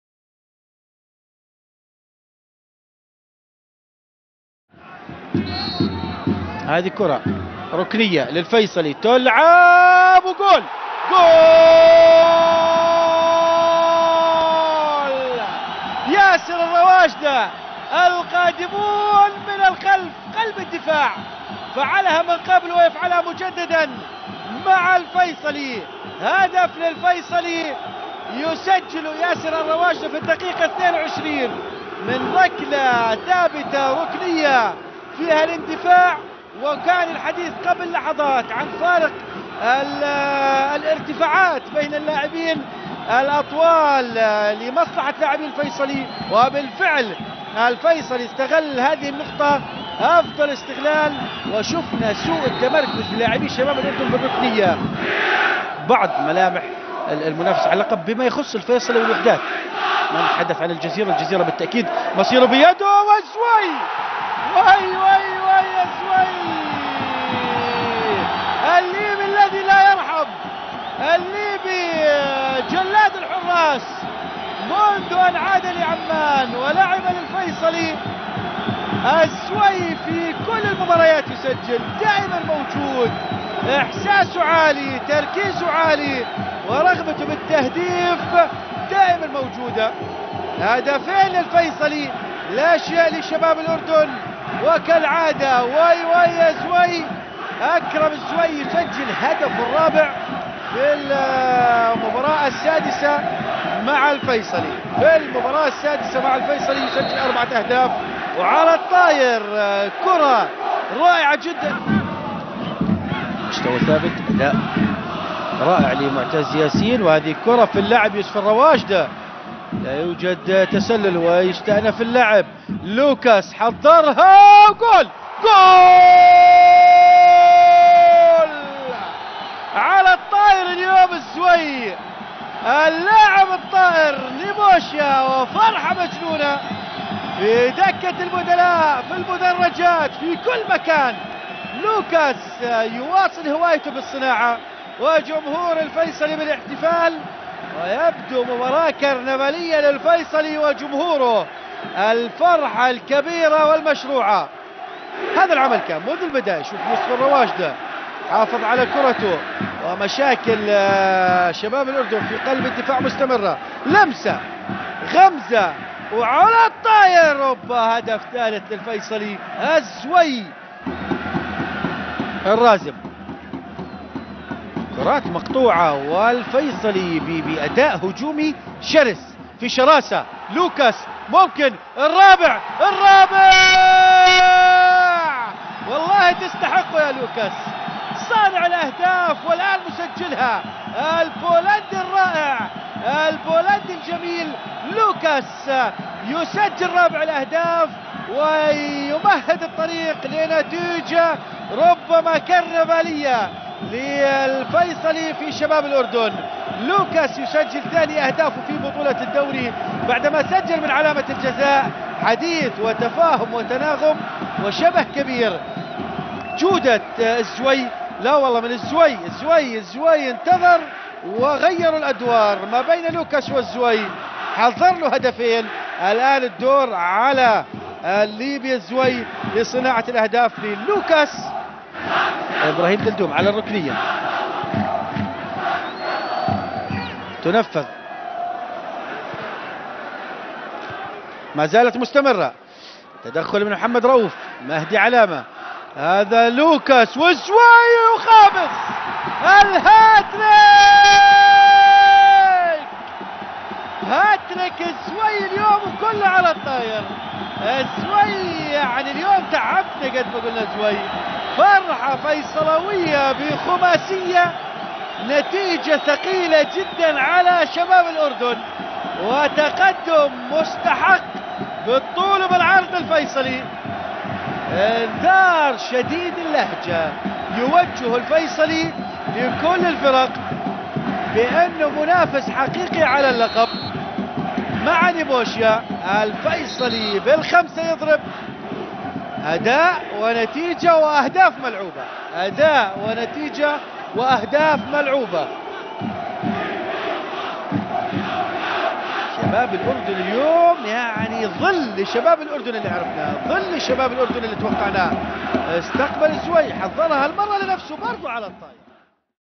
أهداف مباراة الفيصلي 5-0 شباب الأردن | دوري المناصير الأردني 2016/17 الجولة 17
السبت 1-4-2017
Jordanian Football League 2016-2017 Week 17
Faisaly Amman 5-0 Shabab Al Urdun